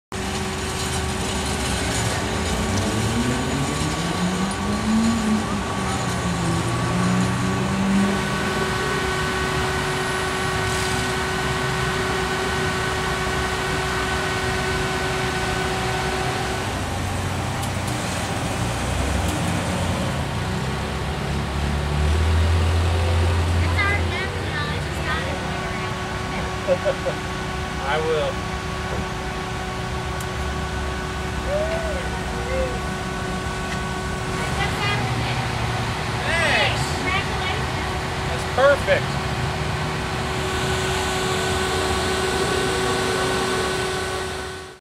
Datsun 240Z
This is a fantastic, nicely-modified Datsun 240Z sporting a V8 that I saw at Road Atlanta during the August 2012 NASA Racing Weekend... such a stunning machine!